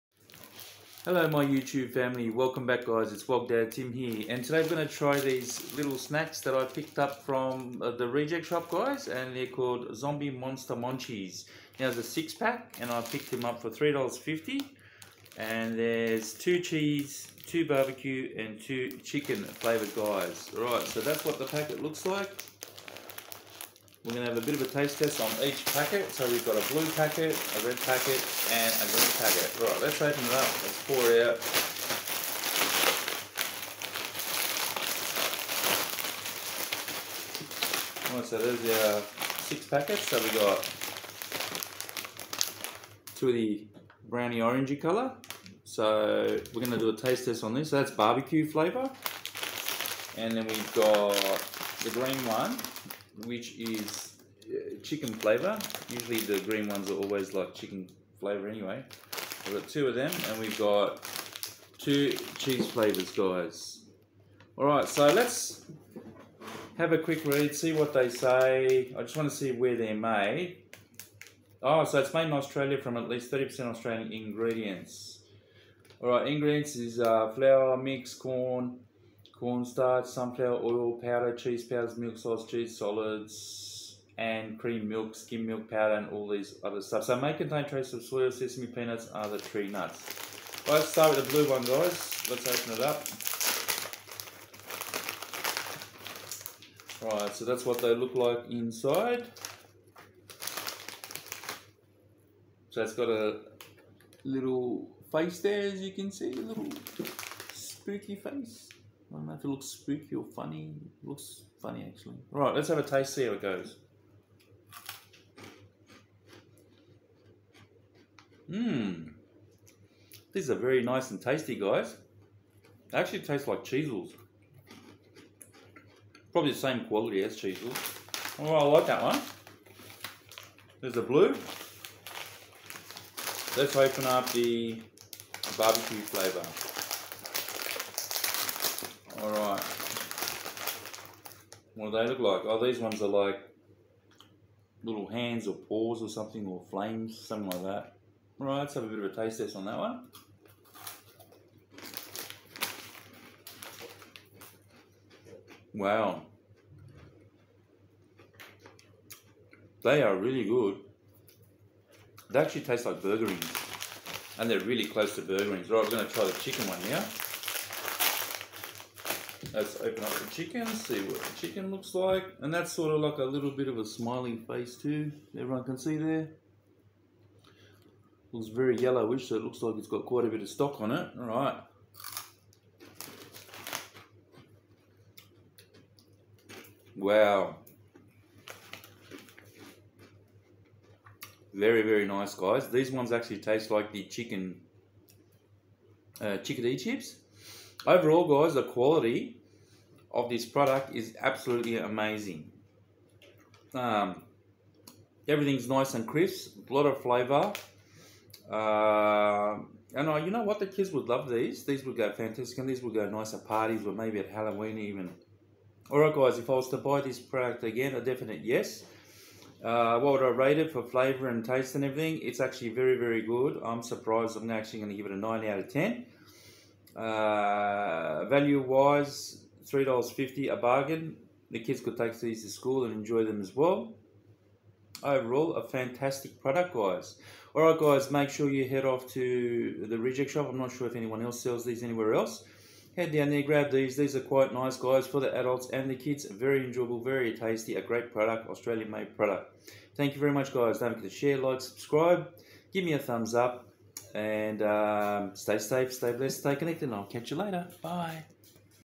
Zombie Monster Munchees Taste Test & Reviews
Hello my YouTube family today another review from the reject shop ,these are very nice and tasty I hope you enjoy my review please subscribe to my channel and stay safe and connected love you all ♥️